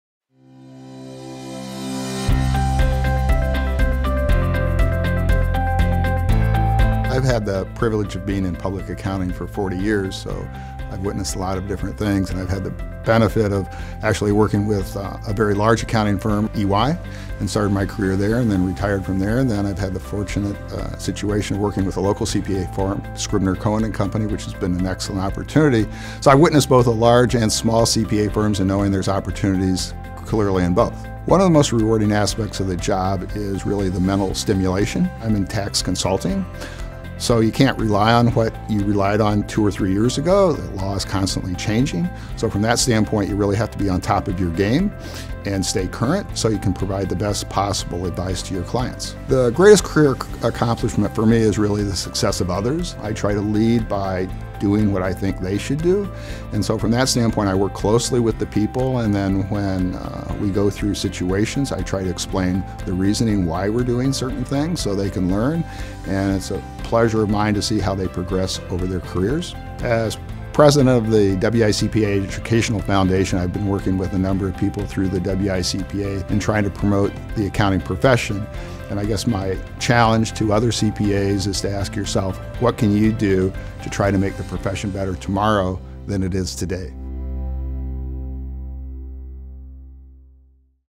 Michael E. Friedman, CPA, JD receives 2019 WICPA CPA in Public Accounting Excellence Award
Michael E. Friedman, CPA, JD was recognized at the 2019 WICPA Member Recognition Banquet & Annual Business Meeting as he received the CPA in Public Accounting Excellence Award.

The CPA in Public Accounting Excellence Award recognizes an individual who is a CPA working in public practice and has demonstrated significant achievements that have contributed to the success and growth of a firm.